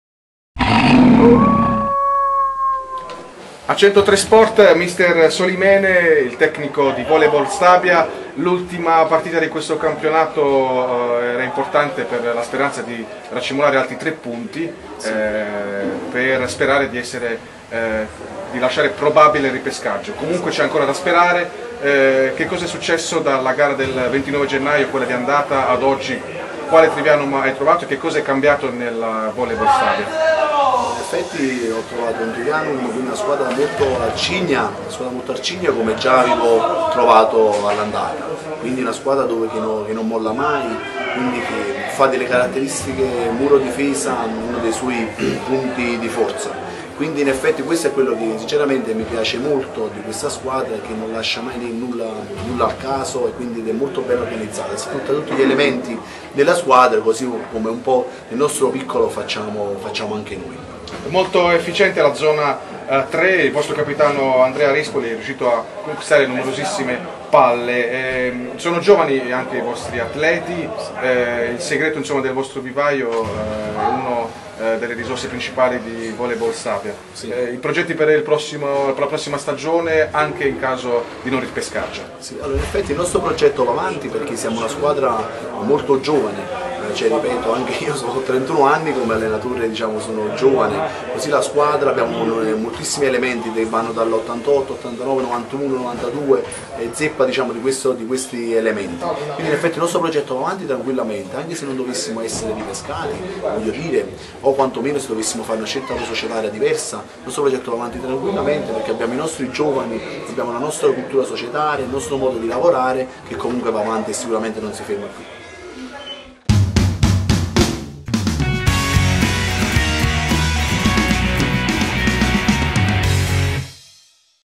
STEFANO SOLIMENE.m4v
TRIVIANUM VOLLEY TRIGGIANO (BA) vs VOLLEY BALL STABIA (Na) 3-1
26° ed ultima giornata del
CAMPIONATO ITALIANO SERIE B2 PALLAVOLO MASCHILE
ITC DE VITI DE MARCO, TRIGGIANO (BA)